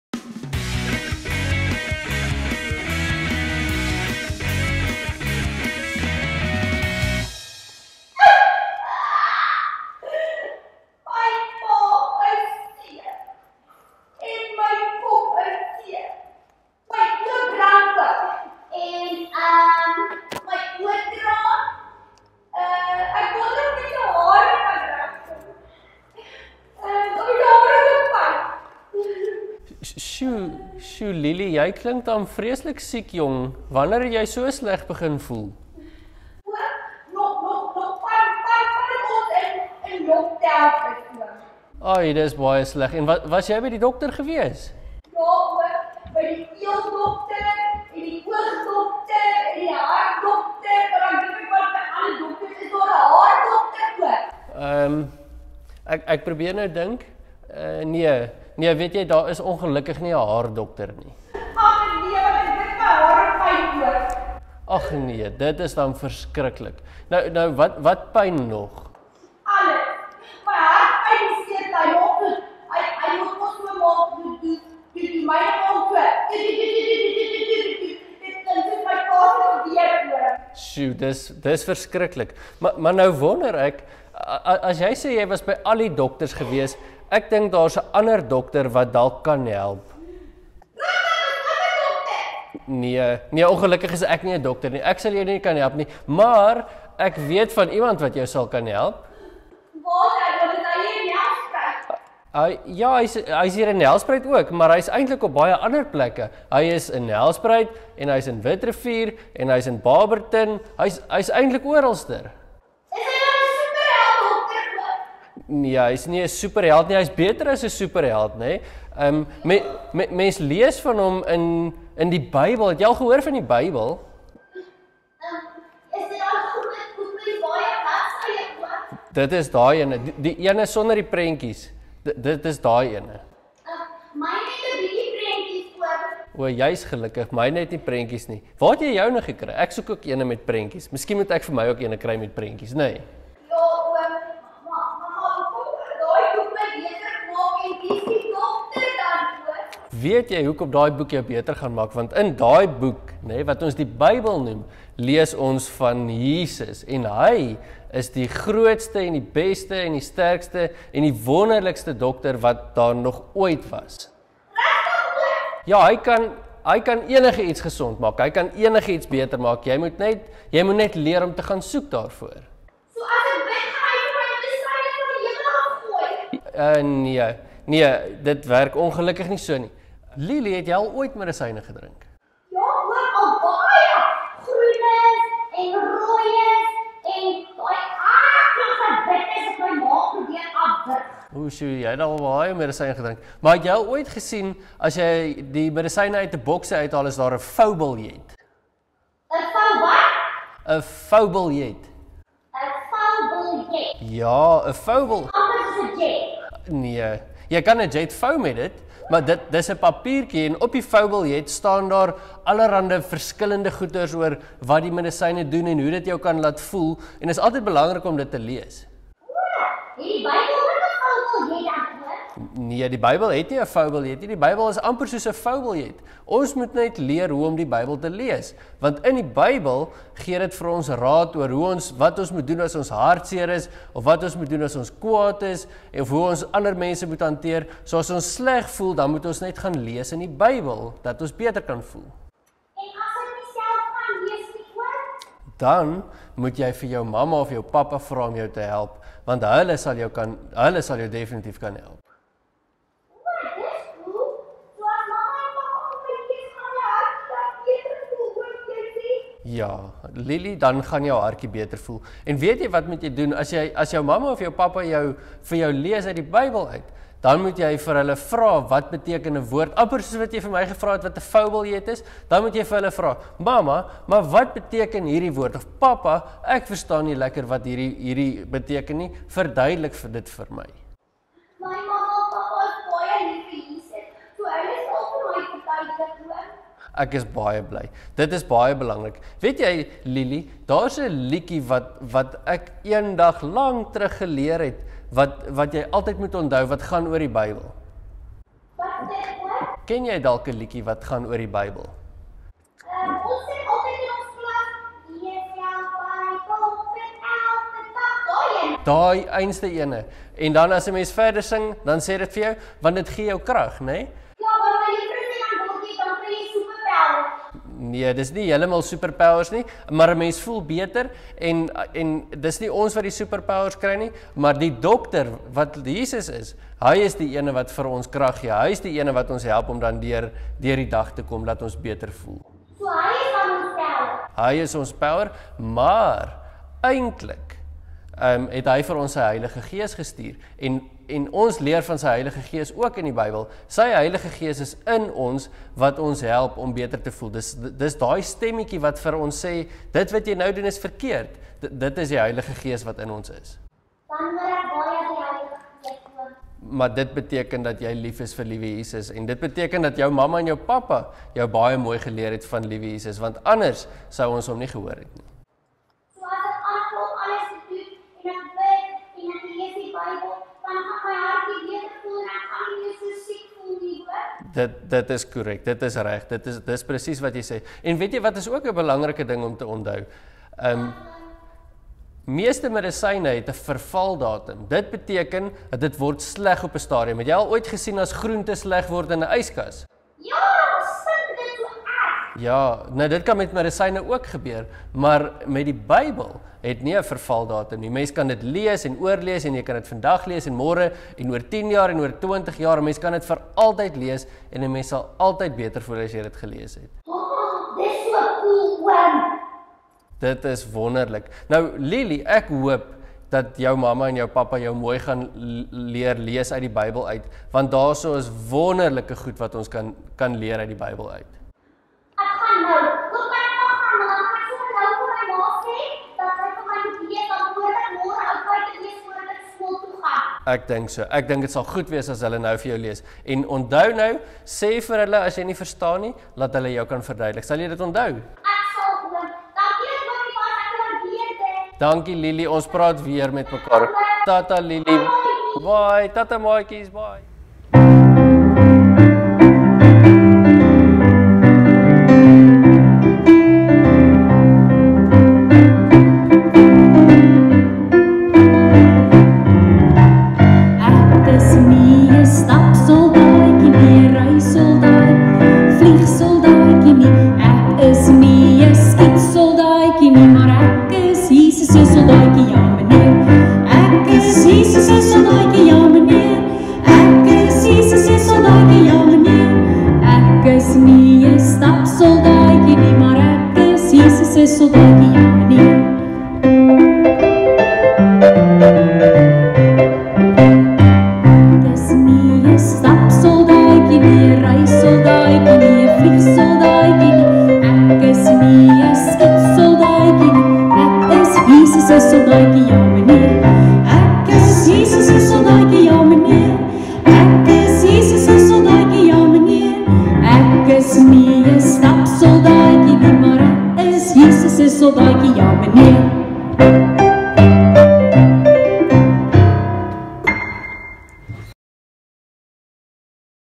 Poppekas 16 Augustus
Ons het 'n nuwe maatjie wat saam met ons gaan leer oor hoe belangrik die Bybel vir ons verhouding met Jesus is. En aan die einde is daar ook 'n lekker saamsing liedjie vir al ons maatjies.